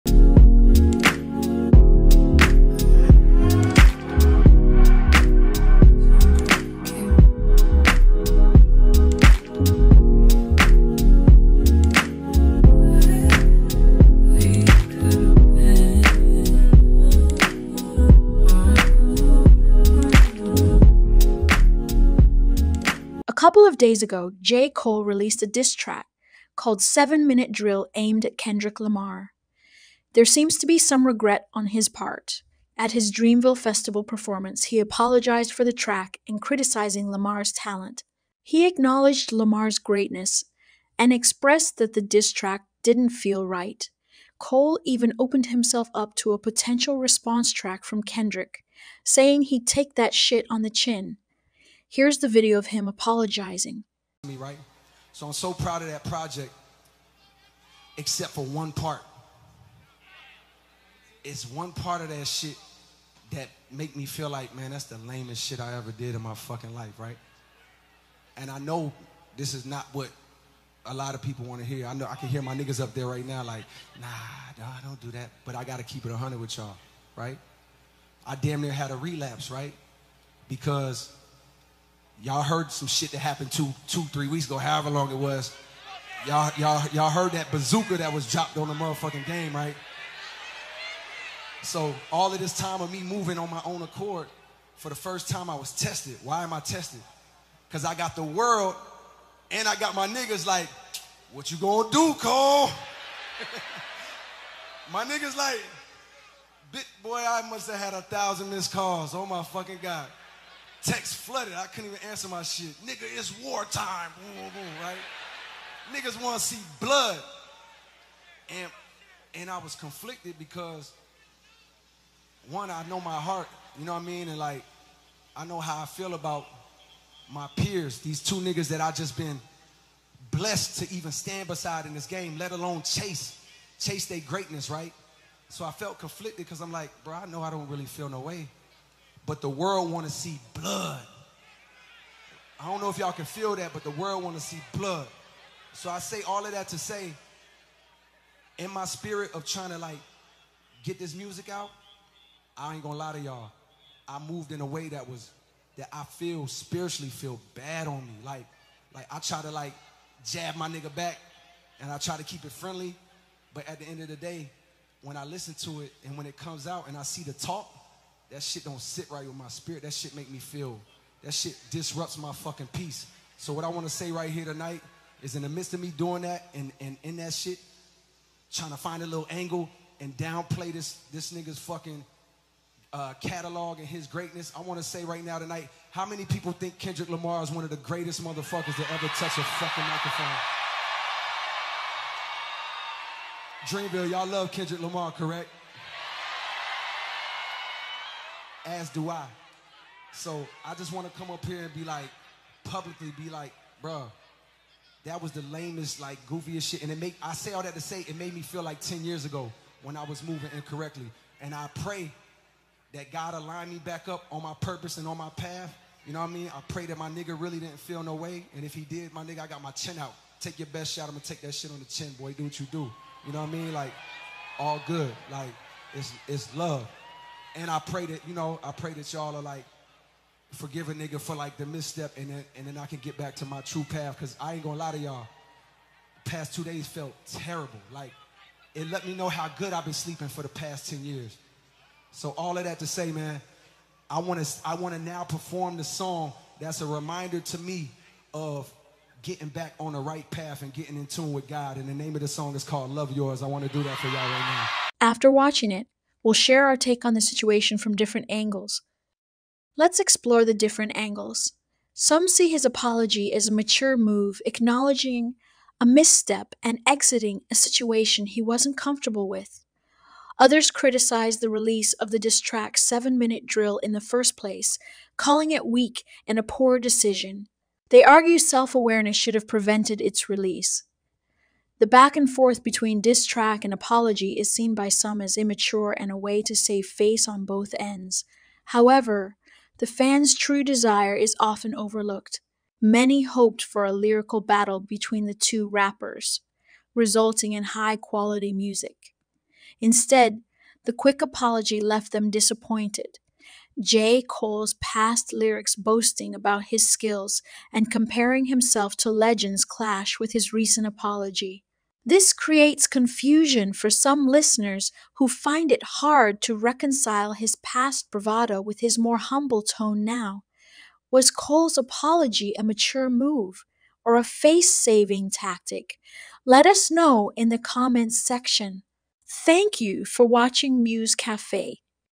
Did J. Cole Diss Himself? Rapper Apologizes to Kendrick Lamar
Hold up, did J. Cole just diss Kendrick Lamar and then take it all back? We dive deep into the surprising back-and-forth between these hip-hop titans. Was it a strategic move or a genuine mistake? Find out what J. Cole had to say about his surprise diss track and why he ended up apologizing to Kung Fu Kenny.